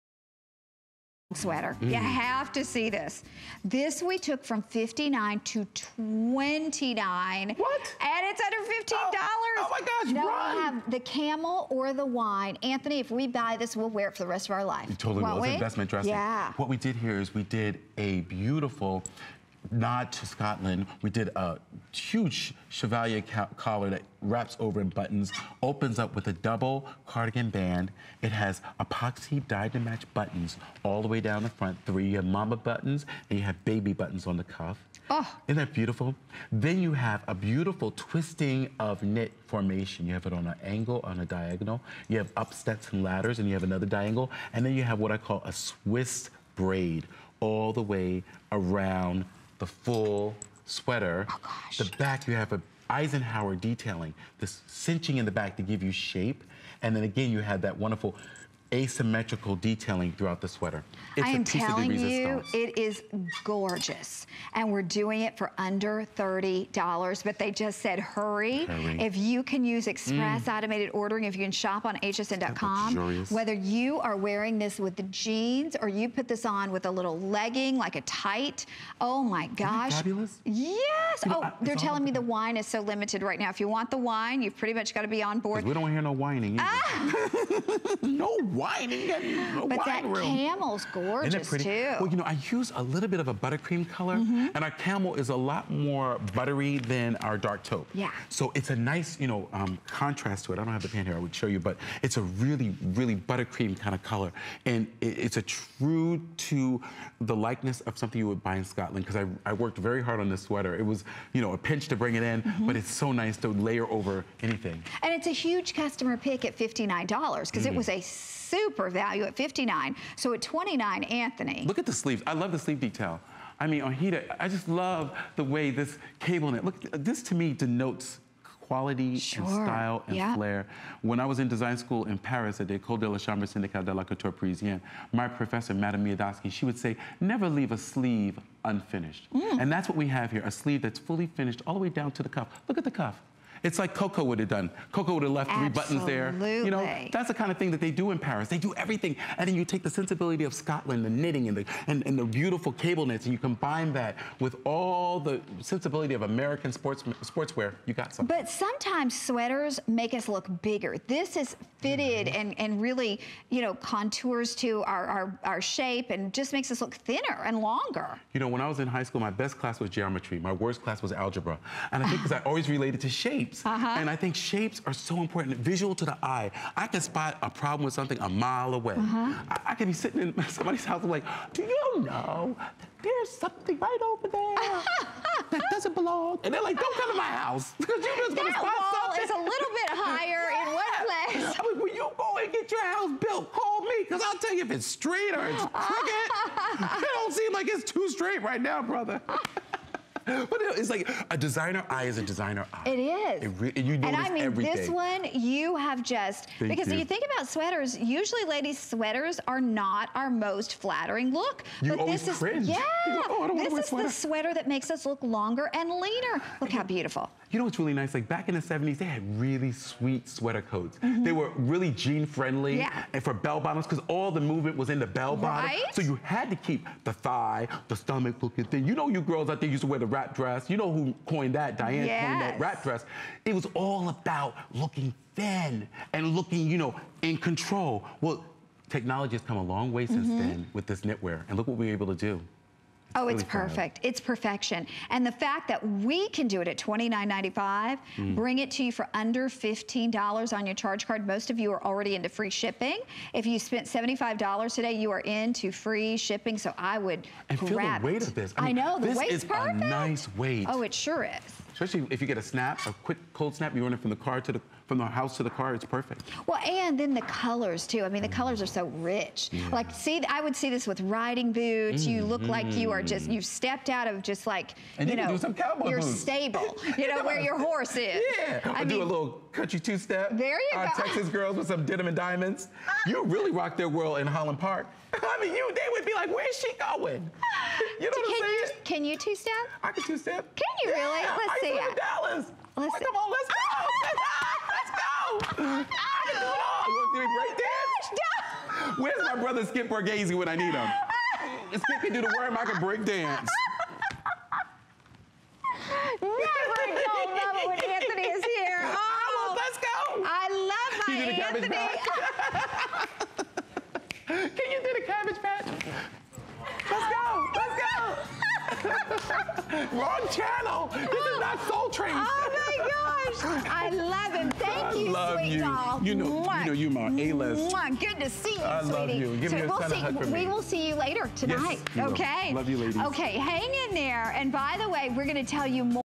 Antthony "Color Me in USA Style" Long Sweater
The comeback knit. Fall for fashion's classic, cozy cardigan all over again. This time around, the onehalf of a twinset is on its own and more...